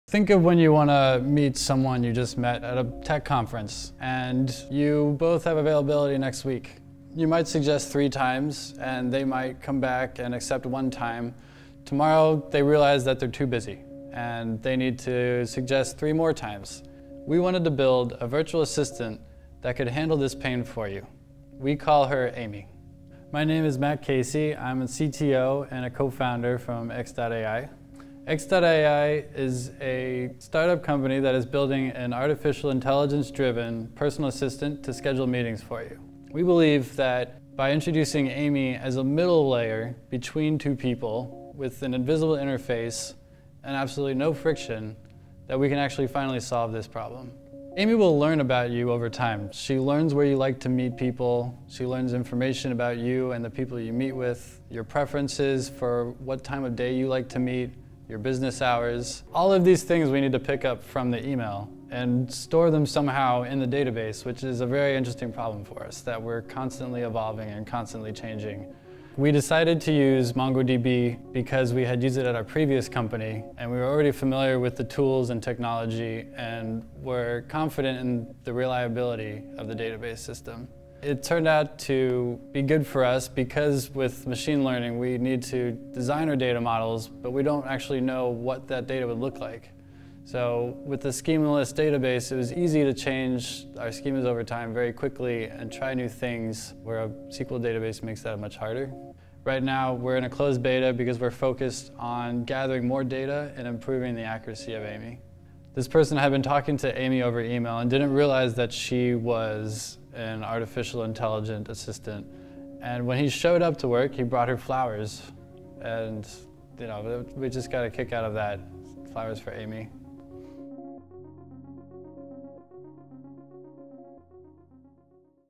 Flowers for Amy: How MongoDB Helps x.ai’s Artificial Intelligence-driven Assistant Appear Human
Amy is an artificial intelligence-powered personal assistant for scheduling meetings. Users can interact interact with her as they would to any other person – and Amy takes care of all the tedious email ping pong that comes along with scheduling a meeting.

Users simply copy their virtual assistant of choice on email with up to four individuals they wish to schedule a meeting, then the assistant takes over and coordinates the schedules using natural language processing, providing an experience that is entirely seamless.

The x.ai team rely on MongoDB, the operational database that powers Amy, to provide the speed and schema-flexibility they require to build the platform. As Matt Casey, CTO and a co-founder x.ai explains: “We didn't actually know what that data would look like, so with the schema-less databases, [it was] easy to change our schemas over time very quickly and try new things.”